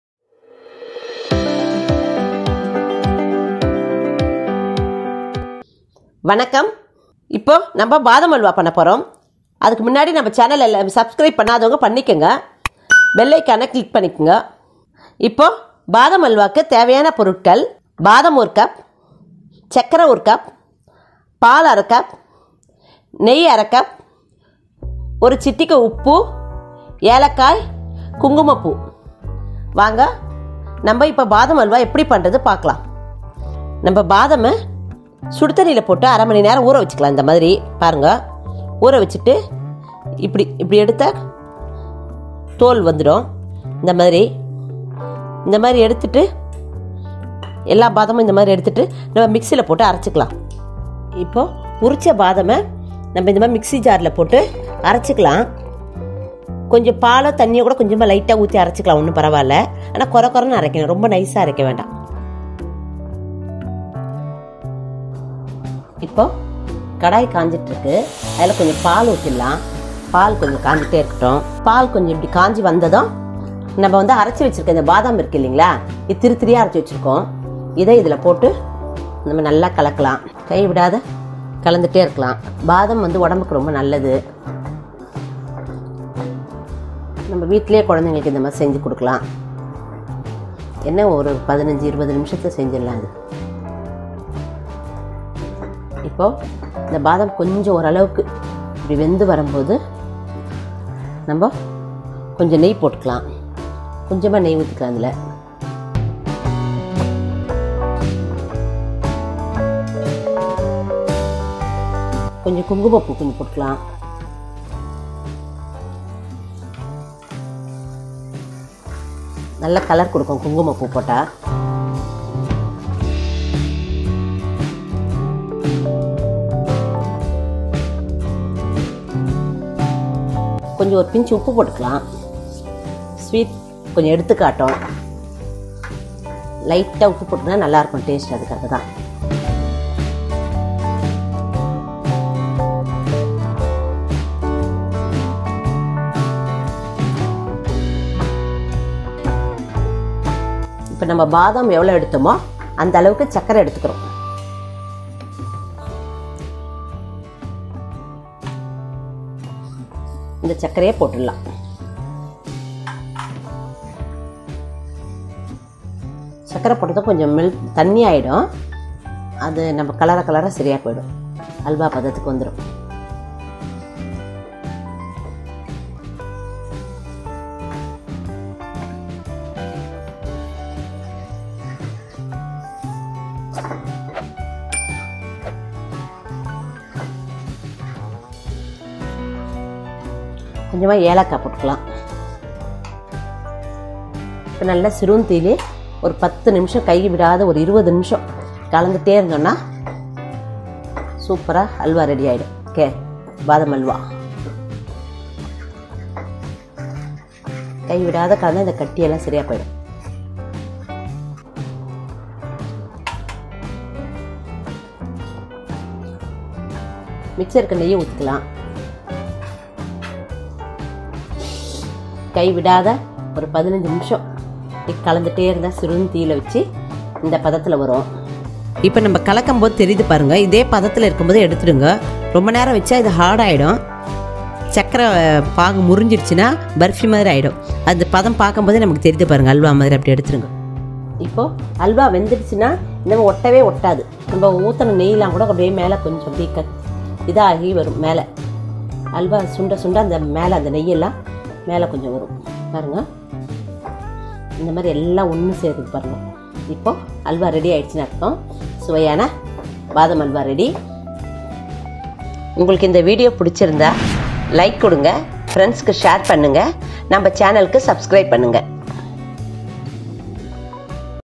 THE KING OF SWEETS "BADAM HALWA" | Quick Sweet Recipe |பாதாம் அல்வா | how to make badam halwa
Try this badam halwa at your home. Badam halwa is quick and easy to make sweet recipe . Give your feedback after trying this recipe at your home.

"THE KING OF SWEETS BADAM HALWA"

Give your feedback in the comment section.